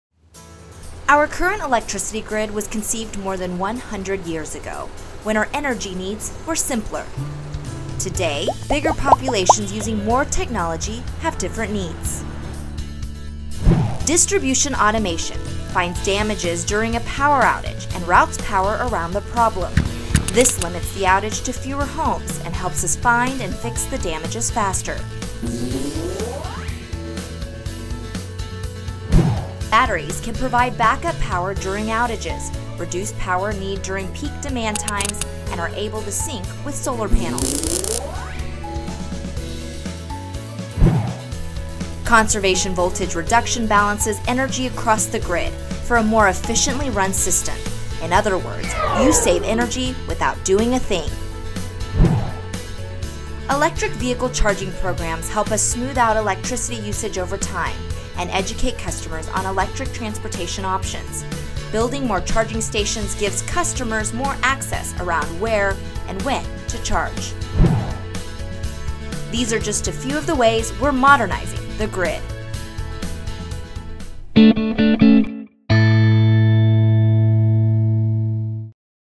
Grid Modernization
What is the smart grid? See how PSE is introducing new technologies that make the grid smarter, greener and more reliable.